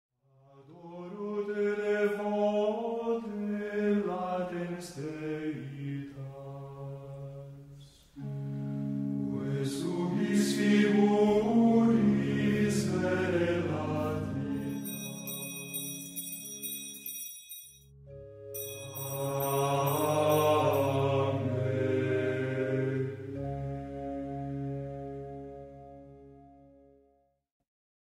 ADORACIÓN AL SANTÍSIMO SACRAMENTO.
Adoración al Santísimo Sacramento. Oración por todas sus intenciones.